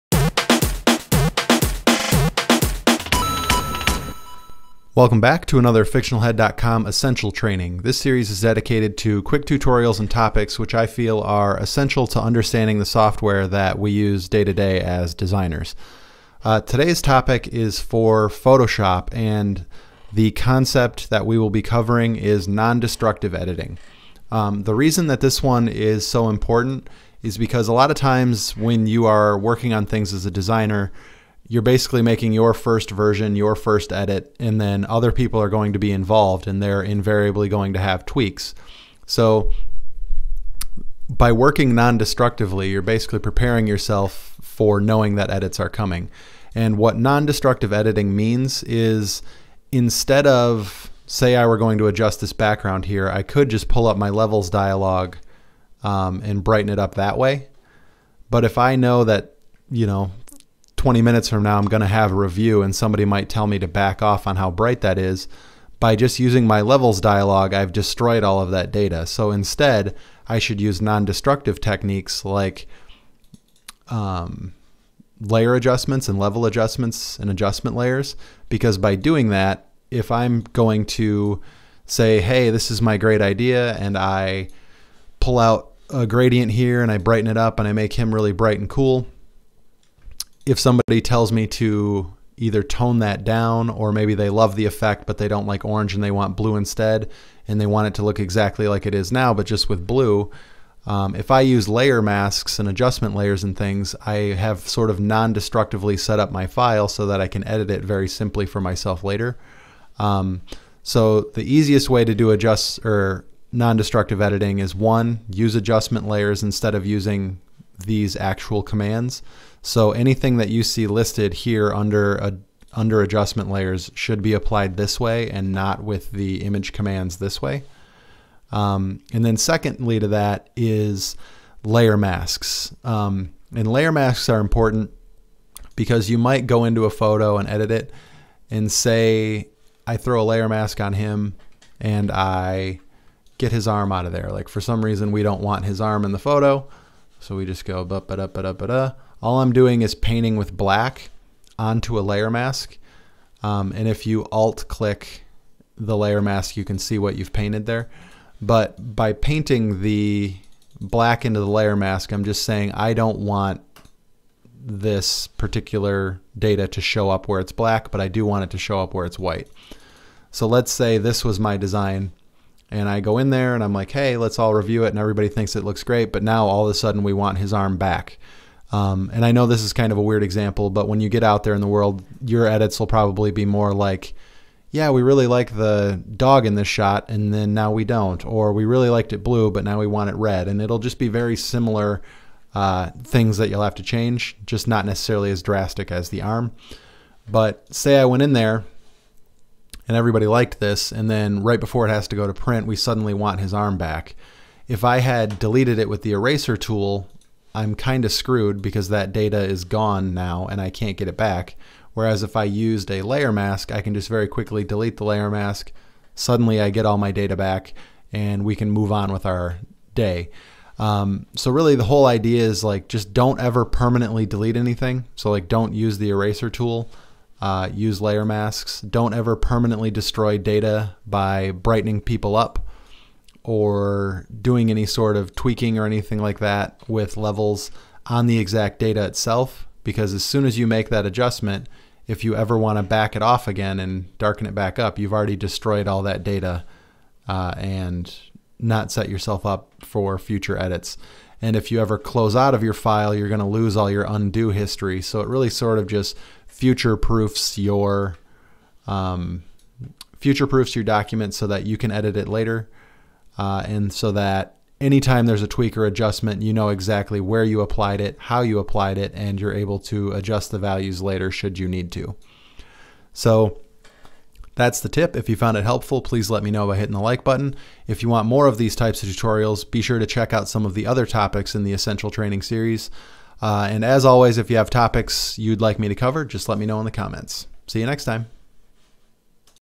Understanding Non-Destructive Editing, Photoshop Essential Training
In this episode, I cover the concept of non-destructive editing, and why it's essential to use when making Photoshop edits.